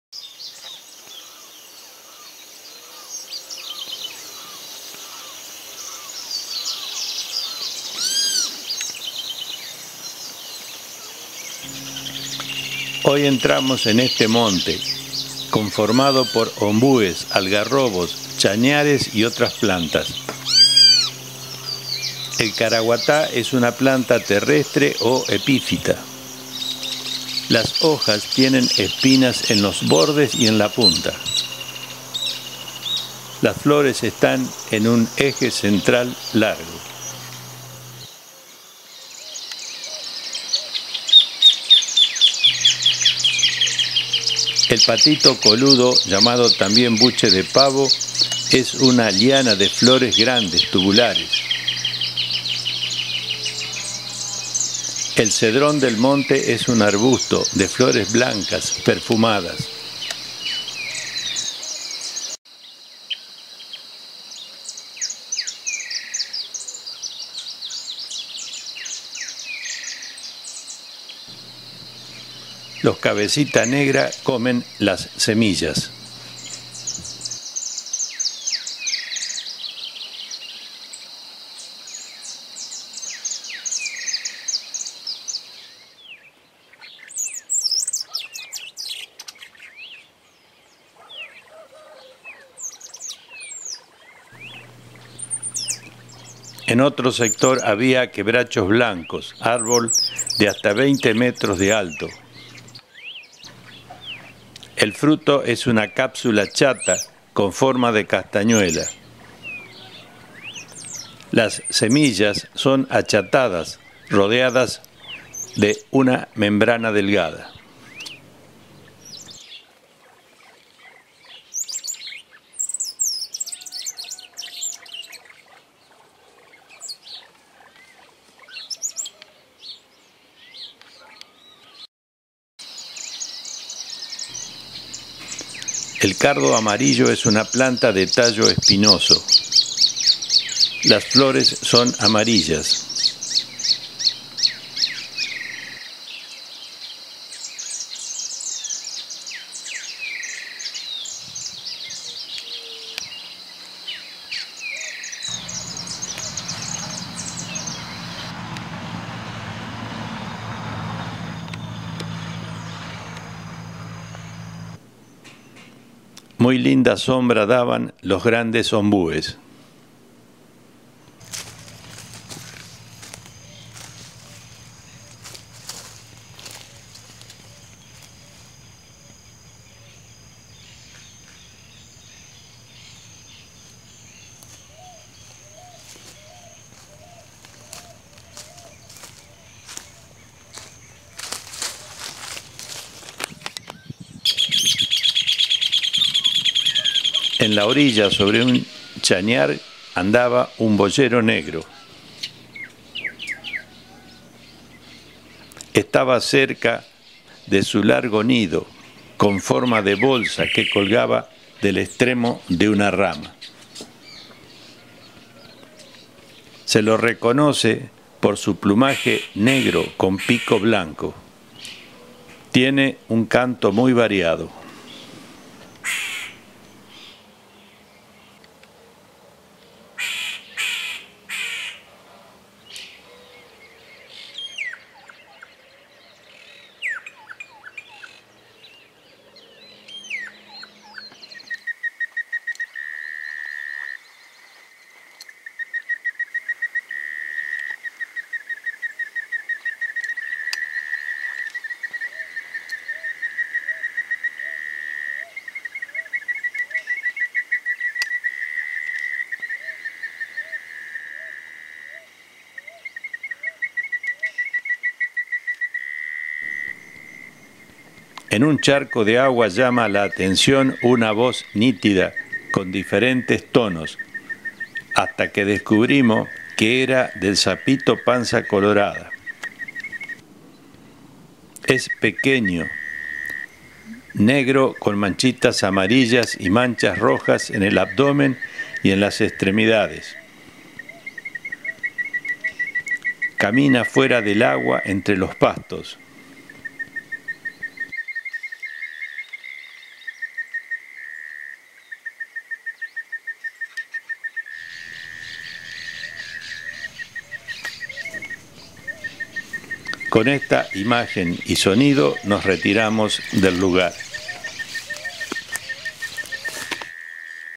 Plantas, aves y ambientes de campos y montes Santa Fe Argentina Parte 3 2023
Caraguatá, Patito coludo, Cabecita negra, Quebracho blanco, Cardo amarillo, Ombú, Boyero negro, Sapito panza colorada